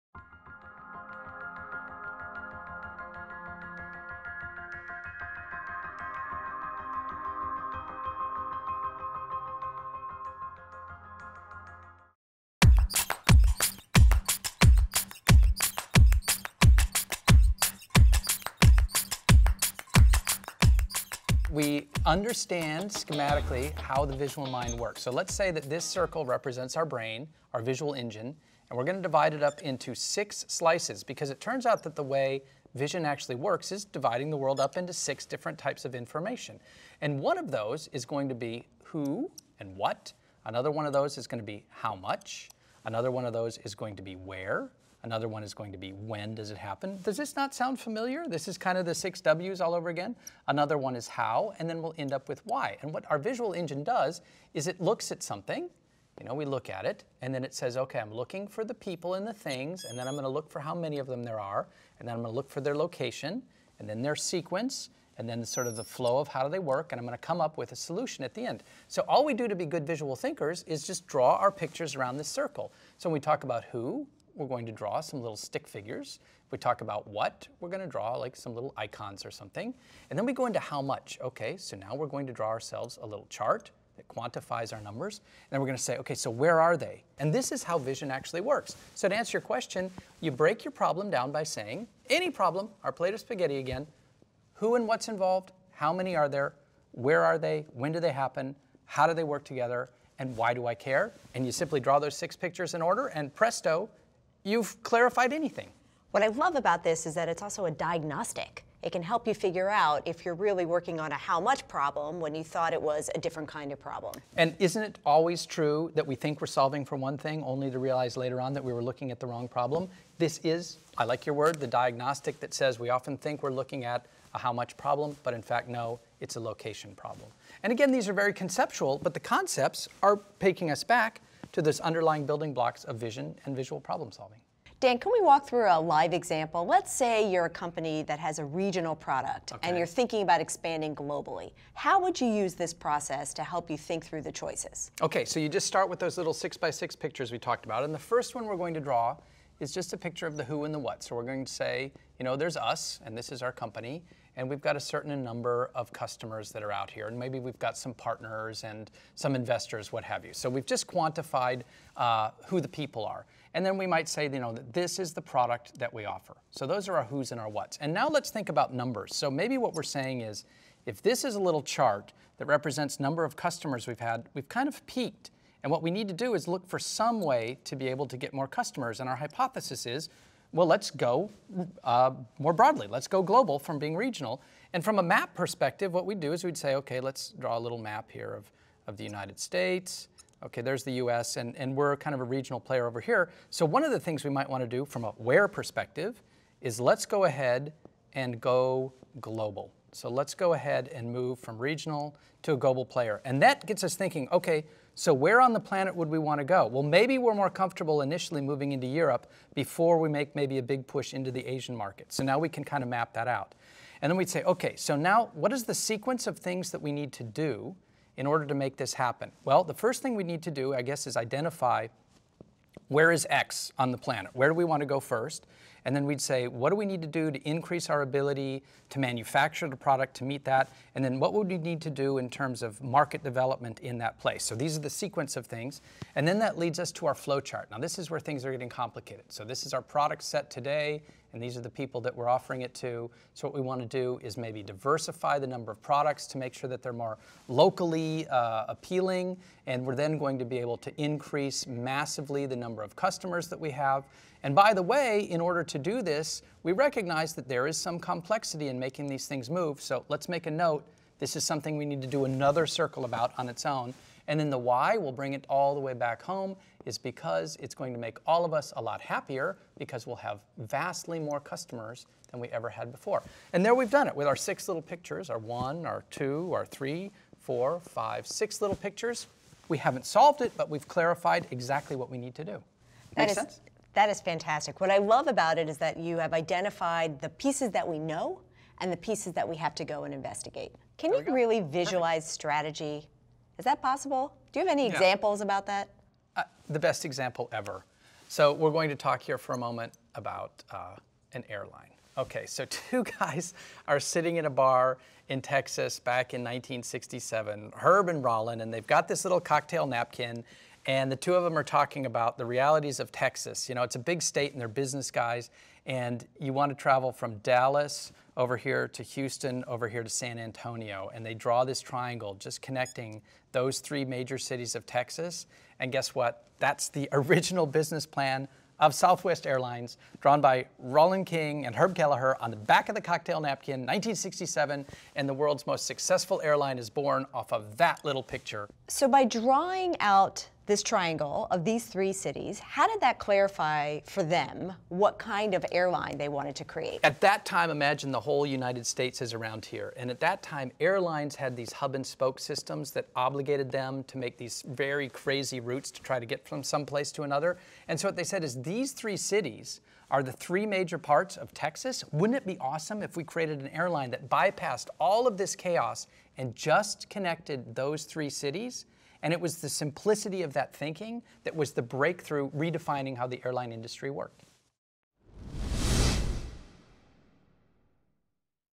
Dan Roam | Problem Solving With Simple Pictures | Singularity University
Lisa Kay Solomon, Chair of Transformational Practices at Singularity University, and Dan Roam, author of "Draw to Win: A Crash Course on How to Lead, Sell, and Innovate With Your Visual Mind," discuss How to Supercharge Problem Solving With Simple Pictures.